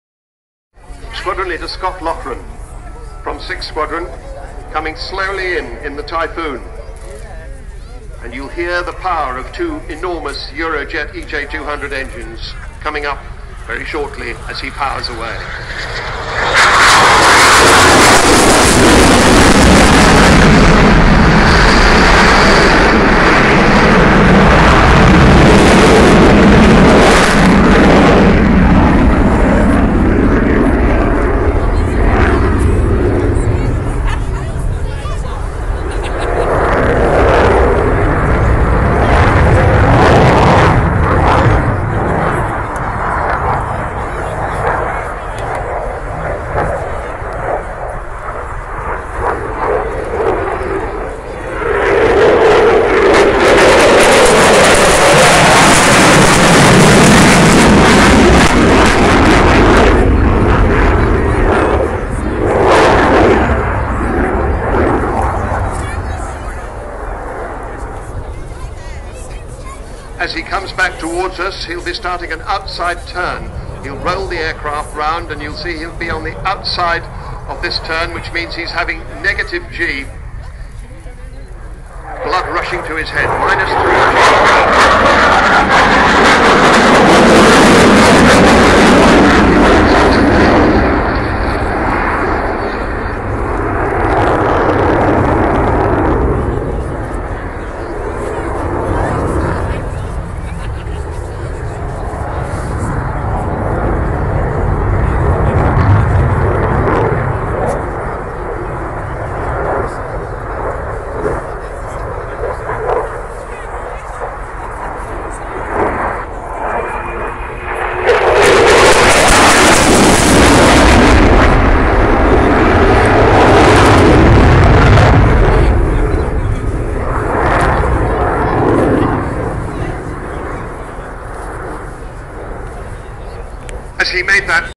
Cosford Airshow 2012 - RAF Typhoon
Cosford Airshow 2012. The RAF Typhoon (not allowed to call it a Eurofighter anymore). A very exciting plane...and just how does the pilot stay conscious through some of these manouevres?